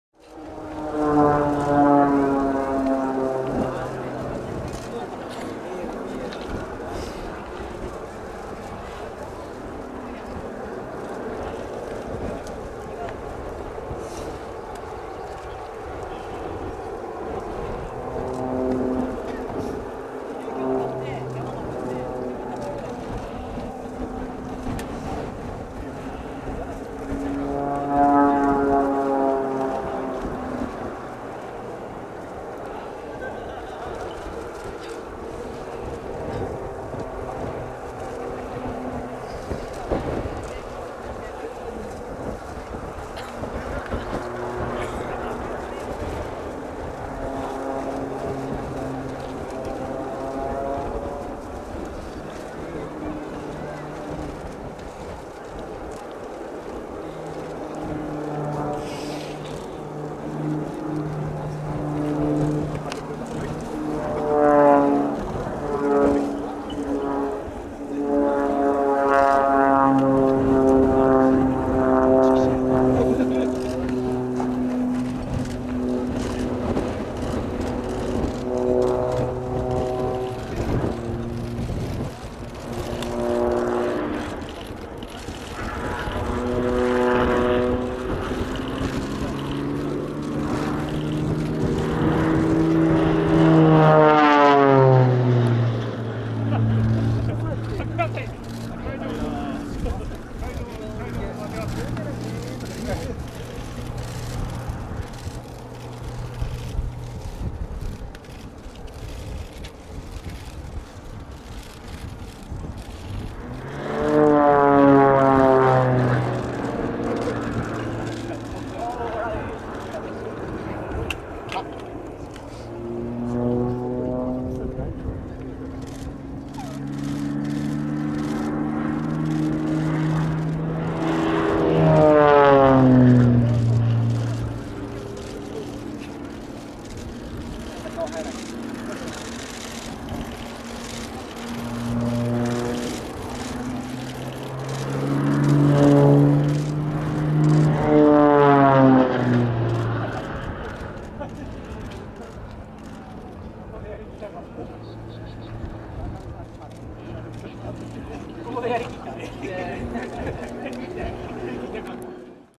本番よりコーフン⁉︎出血大サービス飛行！ウイスキーパパ！岩国フレンドシップデー2018前日予行
岩国フレンドシップデー前日予行でのウイスキーパパによる曲技飛行の一部です。堤防にいる観客のほぼ真上で大サービス飛行をしてくれました！